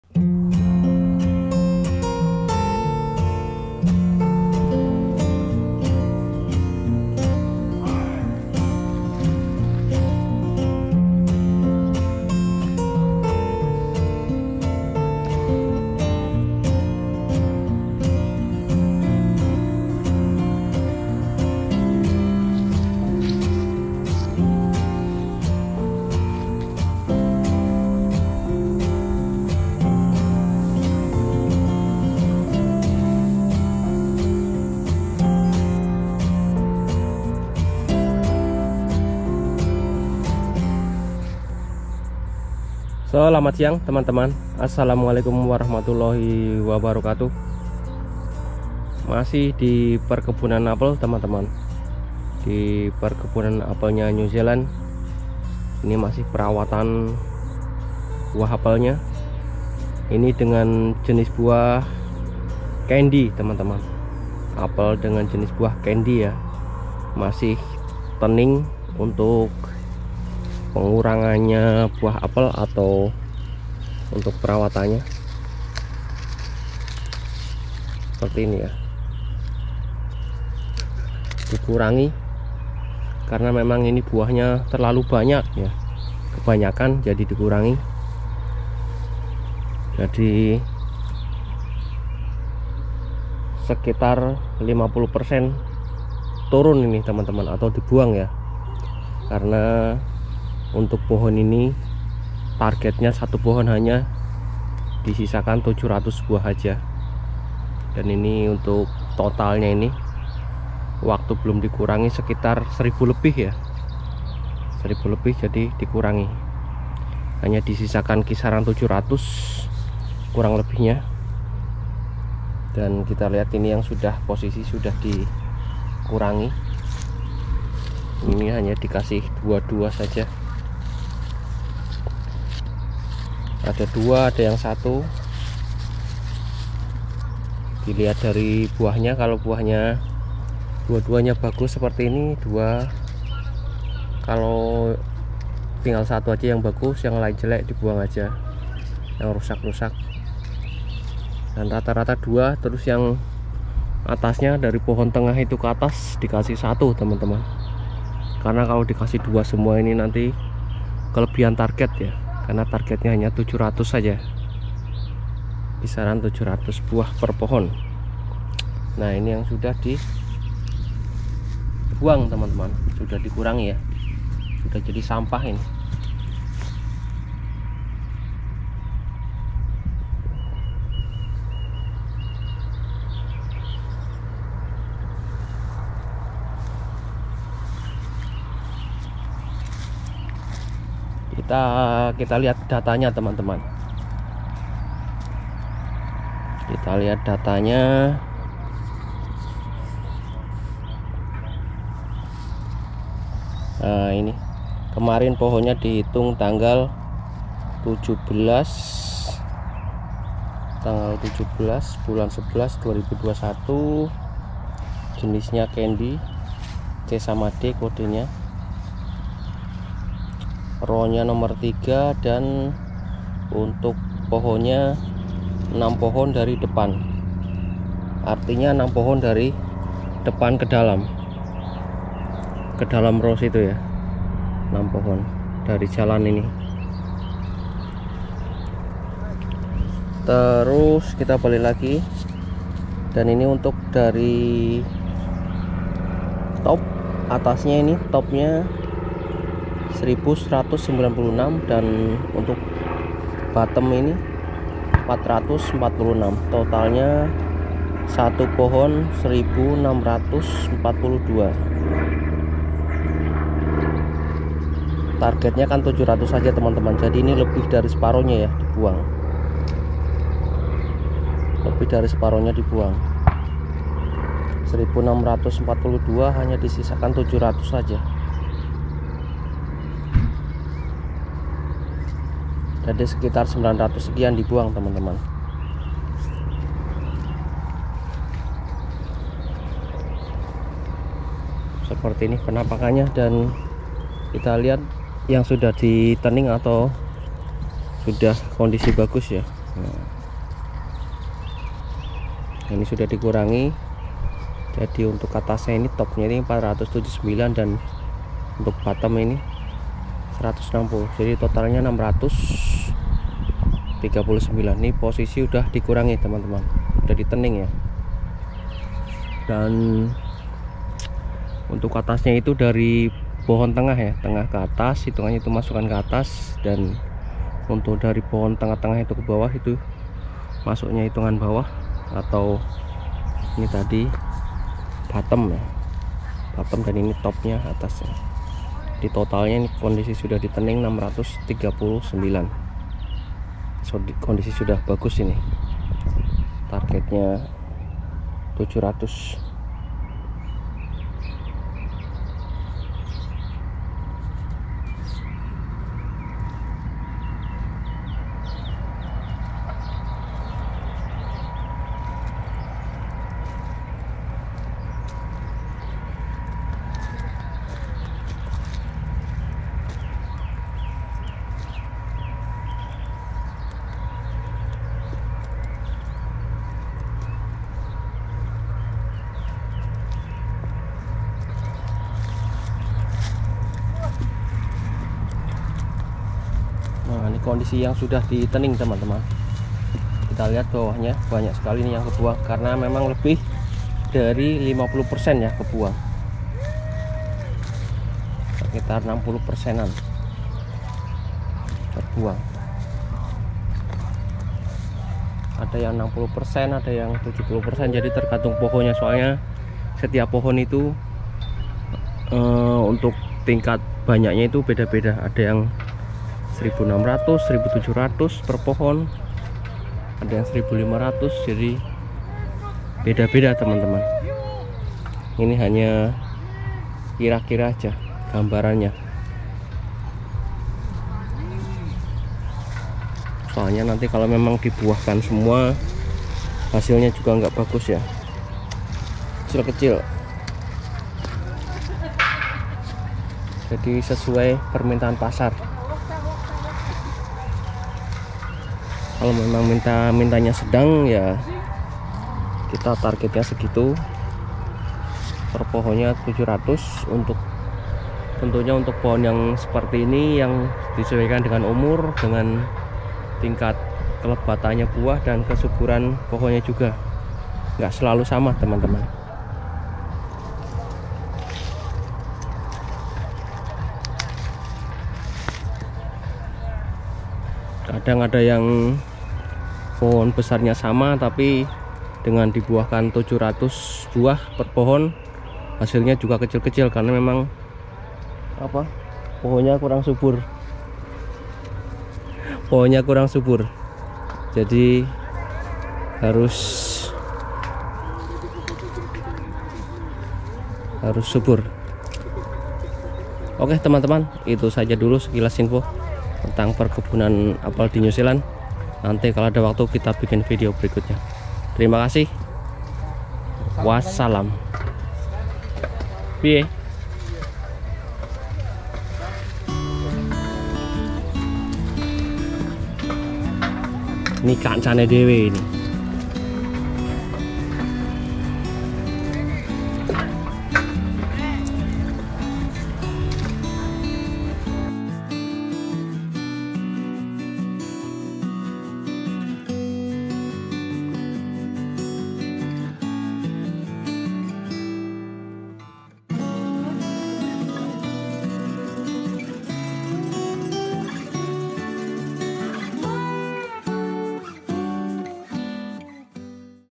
lebih dari 50% terbuang untuk hasilkan apel BERKWALITAS.
Pengurangan apel jenis CANDY di perkebunan apel New zealand.